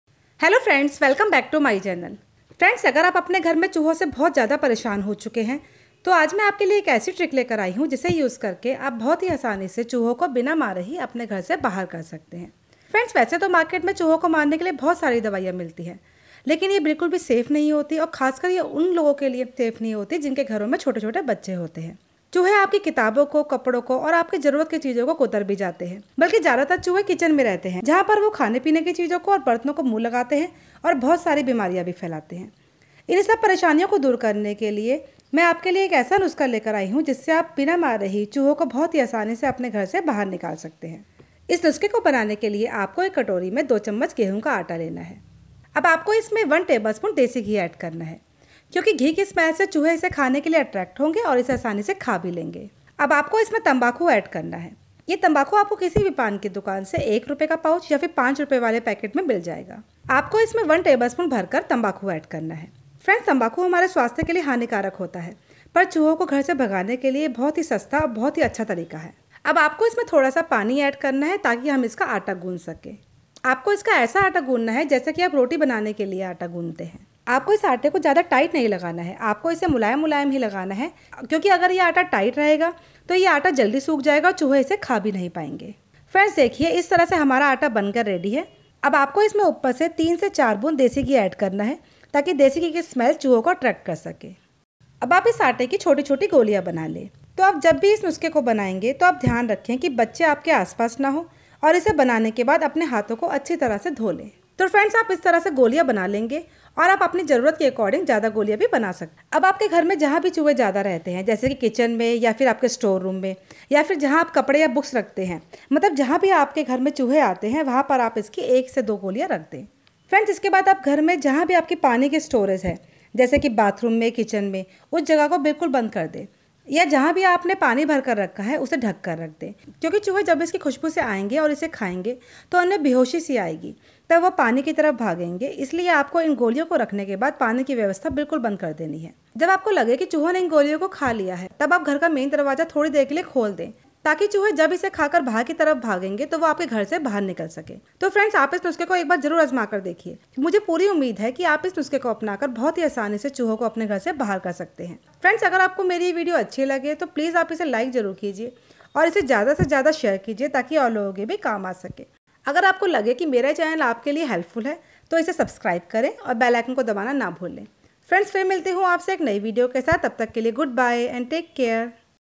बिना मारे ही चूहों को भगाने का तरीका |चूहे घर में दोबारा घुसने की हिम्मत नहीं करेंगे|Get Rid Of Rats
In this video i will share with you home remedy to get rid of rat easily at home.

If you like this video, then please like it and also share it to your friends and relatives.

Ngtips